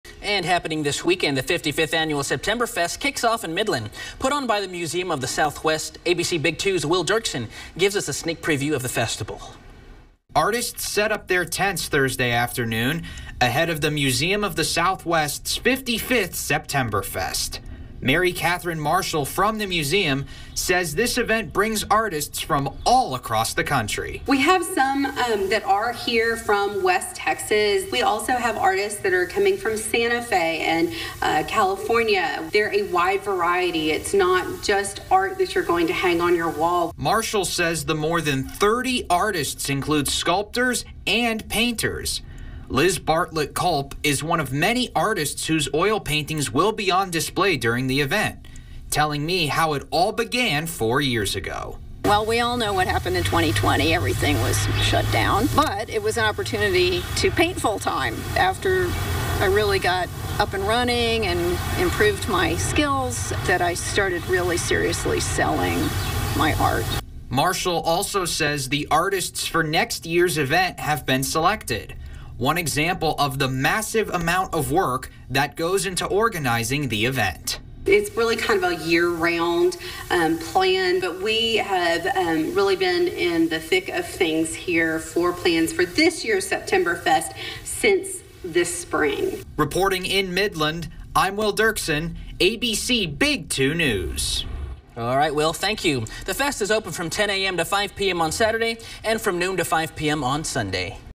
Septemberfest begins at Museum of the Southwest
Artists were setting up their tents Thursday afternoon ahead of the Museum of the Southwest’s 55th annual Septemberfest. Mary Katherine Marshall from the museum said this event brings artists from across the country.